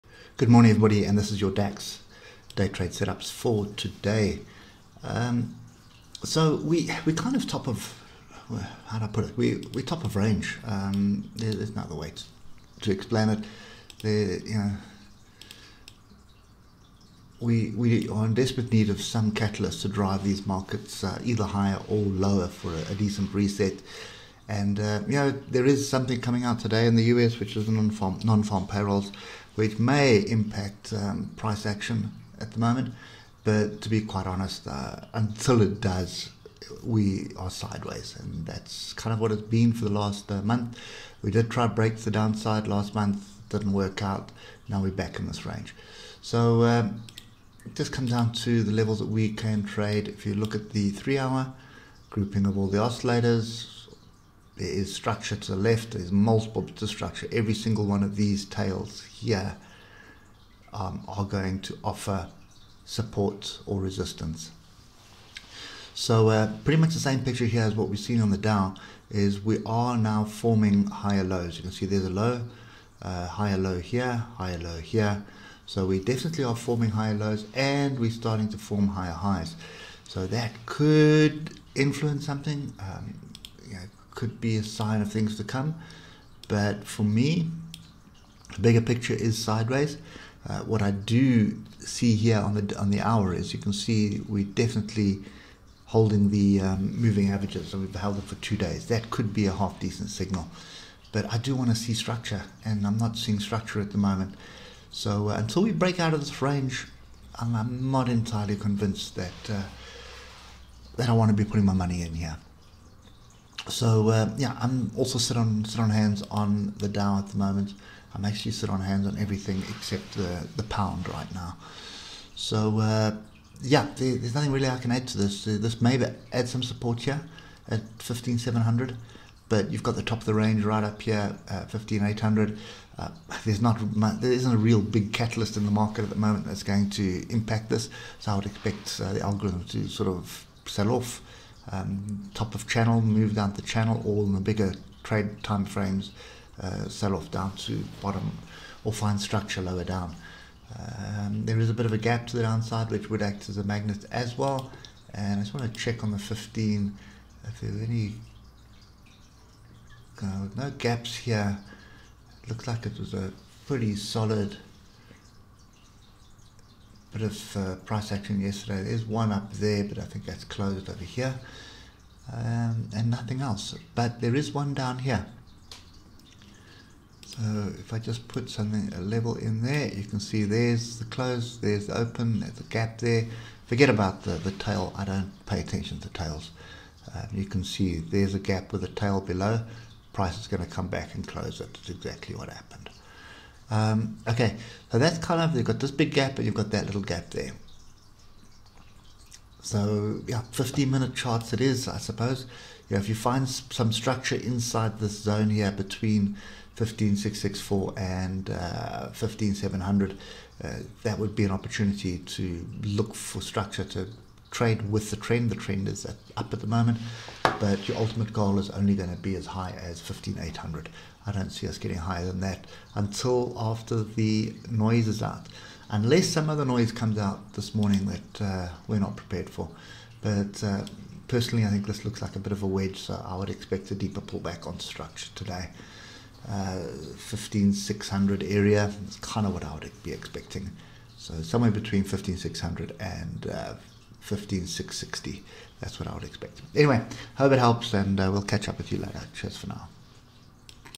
Dax 30 Today 6 August 2021 Technical Analysis. Daily market analysis
Daily Technical Analysis of Dax 30 Today 6 August 2021
Daily market analysis of the DAX 30, or DAX Index, is a German stock market index comprising the 30 biggest companies - by market capitalization and liquidity - trading on the Frankfurt Stock Exchange (FSE). The DAX 30 is considered a strong measure of German and European economic health.